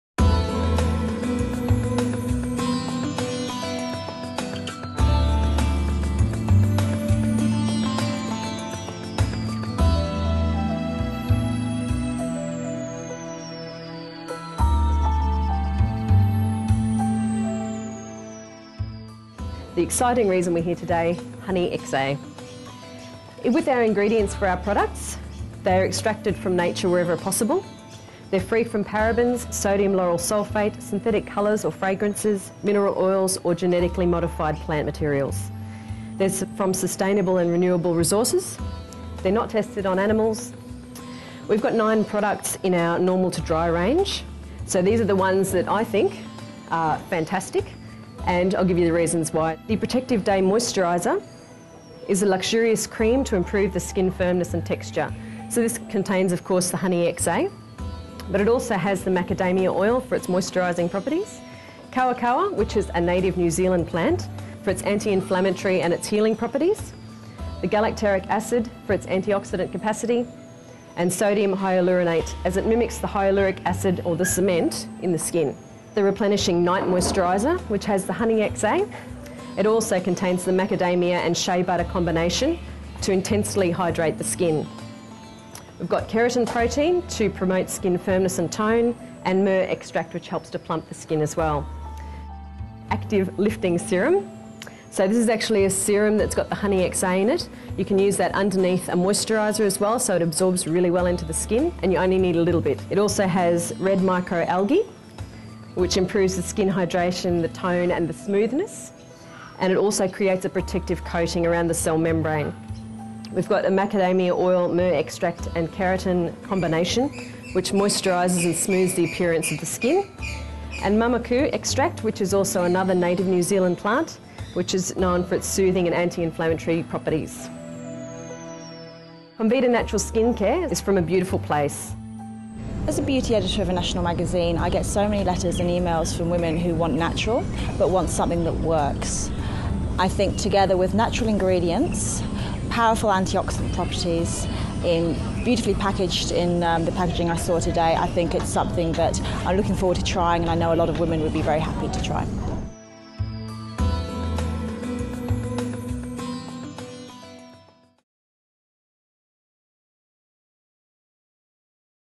Comvita HuniXA Skincare - Normal to Dry Range
Comvita Australia share their top picks from the new HuniXA natural skincare range, in Normal to Dry, using a unique formulation of Comvita Manuka Honey.